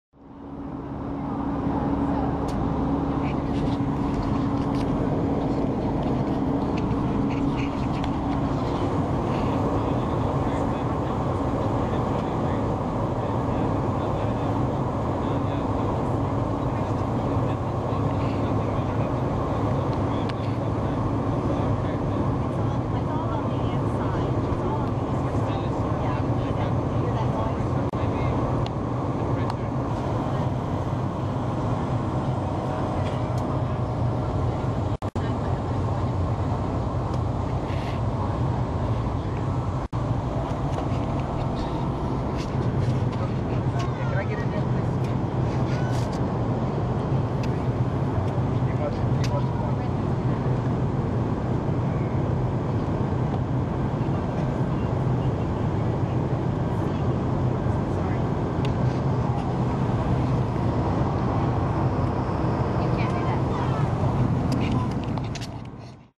Plane panel cracks mid-flight forcing emergency landing
An American Airlines flight heading from San Francisco to Dallas made an emergency landing when the cabin's wall panels cracked loose on Monday.